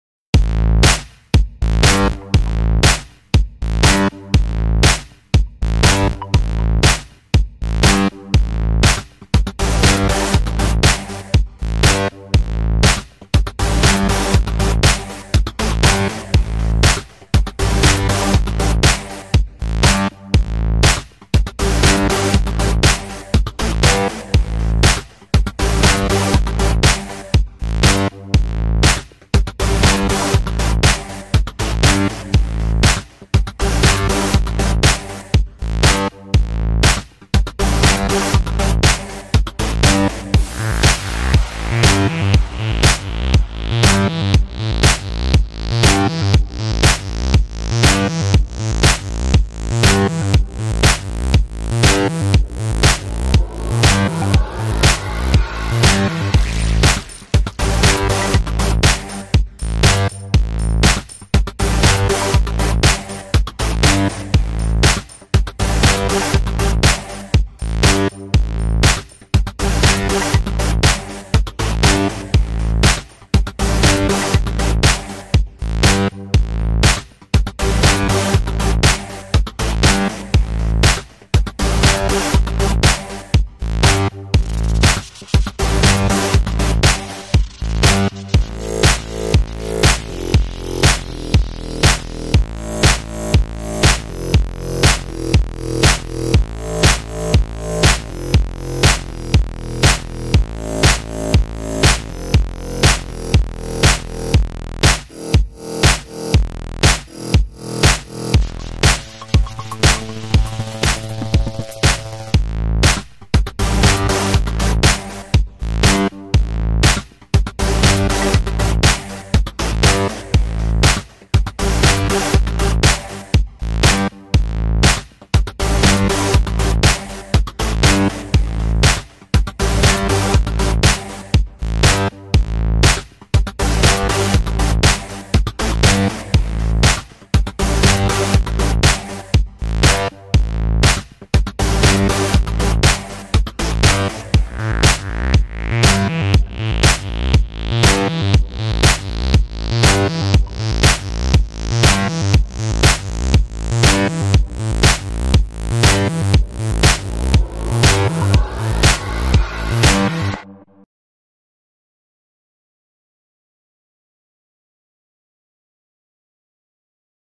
E1 : NAIROBI MINI 3 PRO SKYLINE
DJI Mini 3 Pro, video shooting in Nairobi Westlands, Kenya